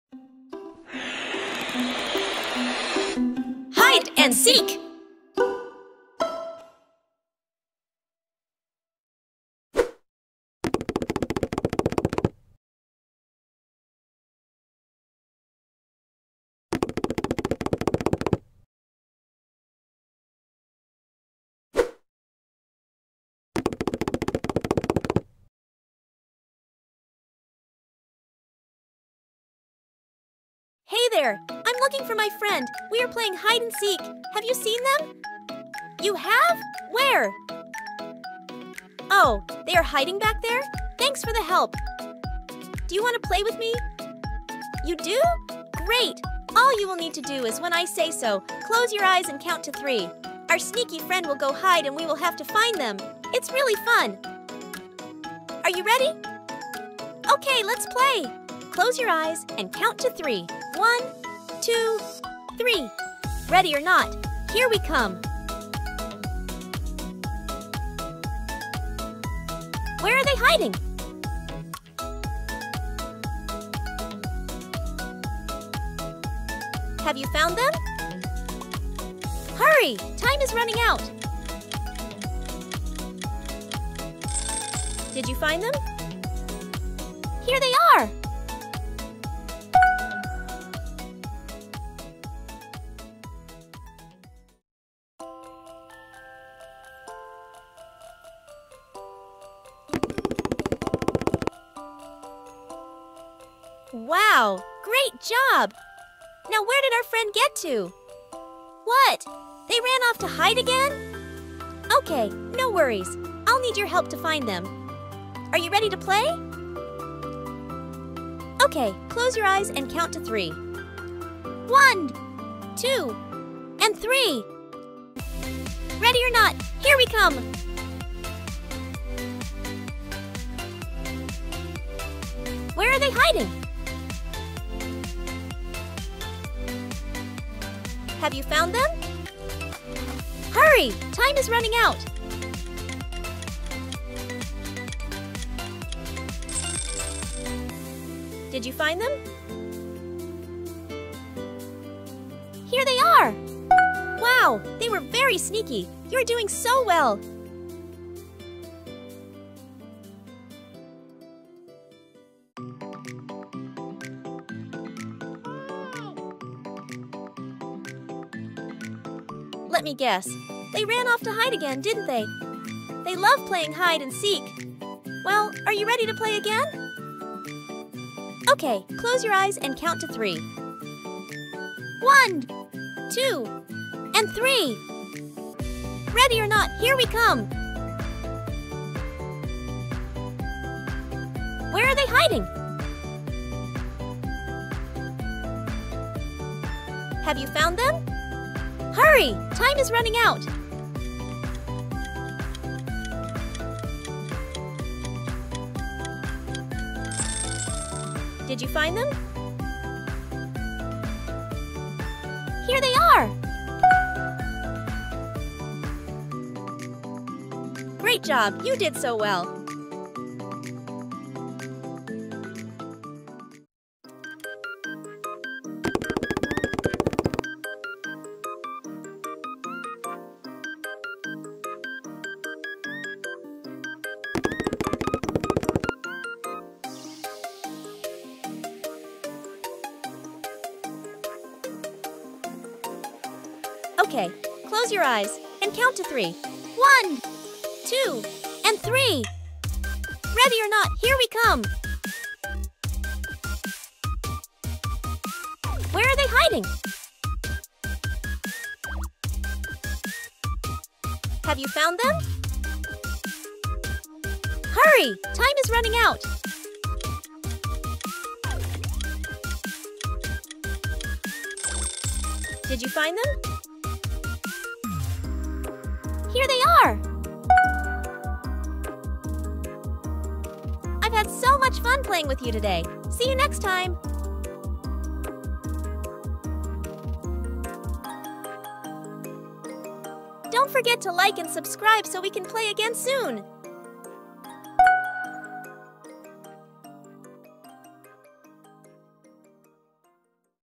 Find Blippi! Fun Hide and Seek Adventure for Toddlers | Educational Game for Kids
Join the fun in this exciting hide and seek adventure with Blippi, perfect for toddlers aged 2 to 5 years old! In this engaging and educational mini-game series, your little ones will have a blast searching for their favorite character, Blippi, hidden in colorful and vibrant scenes. This interactive game helps develop attention to detail, problem-solving skills, and keeps kids entertained for hours.

Key Features:

Fun and educational hide and seek game for toddlers
Colorful and vibrant scenes filled with various objects
Helps develop attention to detail and problem-solving skills
Perfect for children aged 2 to 5 years old
Featuring the beloved character Blippi